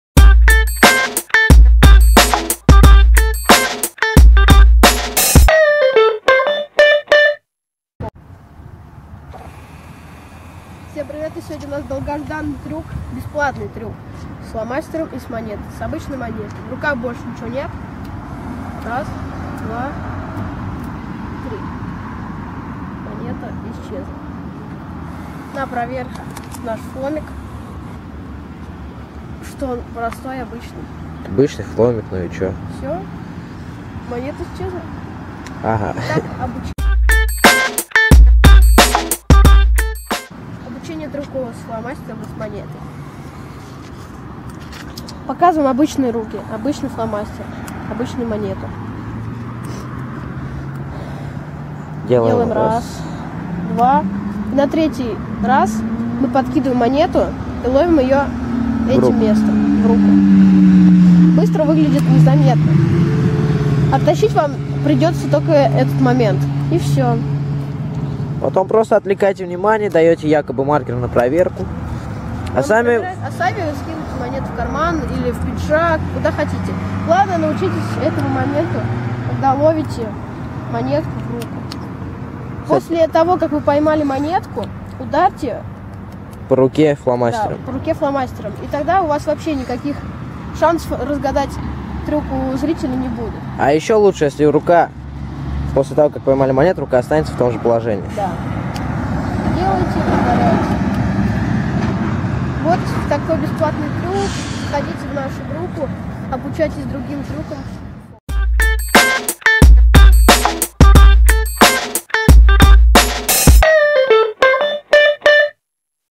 Обучение фокусу "Монета в колпачок"
Цена: Бесплатный
Описание: Классный фокус. Очень быстрый фокус. Который можно показывать где пожелаете!